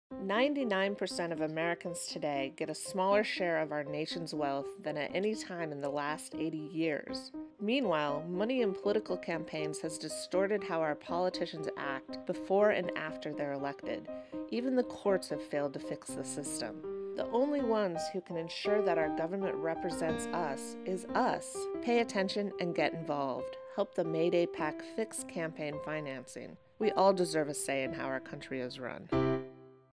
We all deserve a say!
A 30-second video created for mayday.us MAYDAYin30 Video Contest.

and vote for this video, thanks!!!